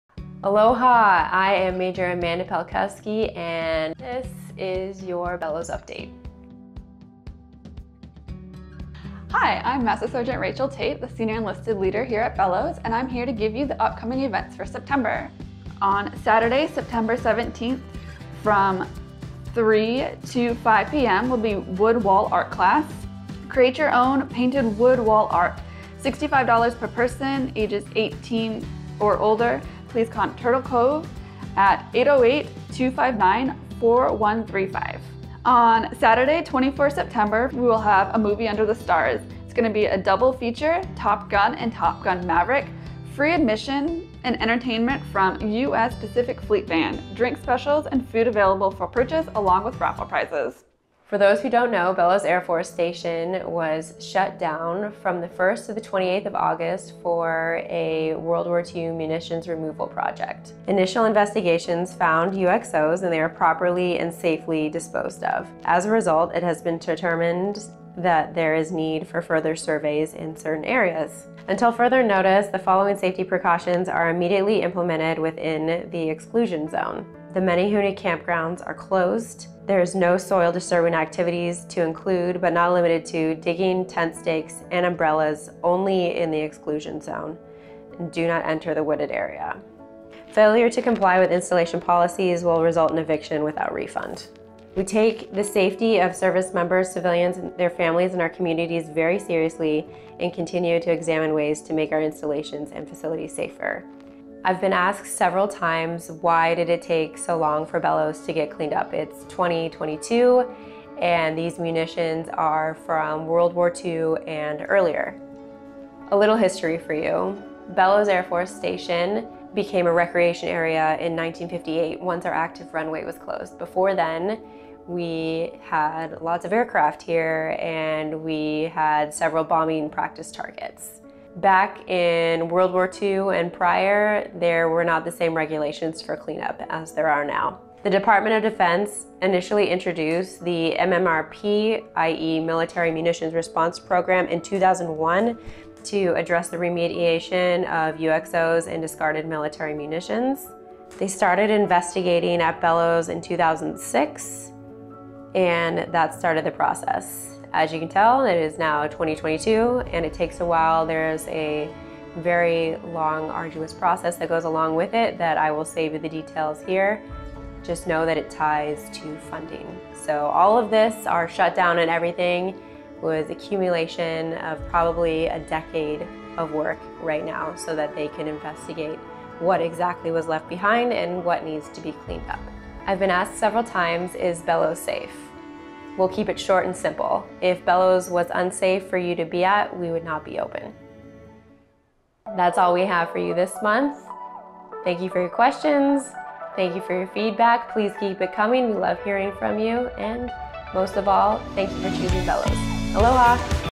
Commander Update: September 2022
September 2022 Updates:
• Why did it take so long to survey Bellows AFS?
• Are we safe at Bellows AFS?